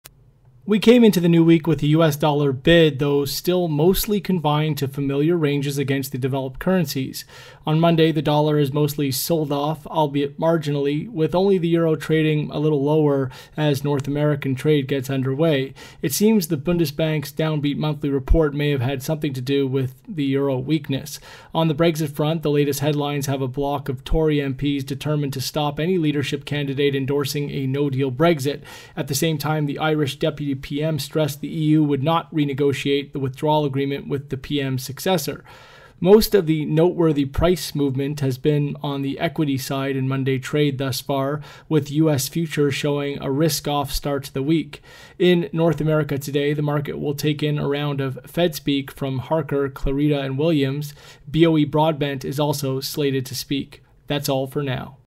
Dollar Unwilling to Extend Run, Stocks Falter
We came into the new week with the US Dollar bid, though still mostly confined to familiar ranges against the developed currencies. On Monday, the Dollar has sold off, albeit marginally, with only the Euro trading a little lower. But FX has been quiet and it’s the move lower in stocks that’s been the real attention getter.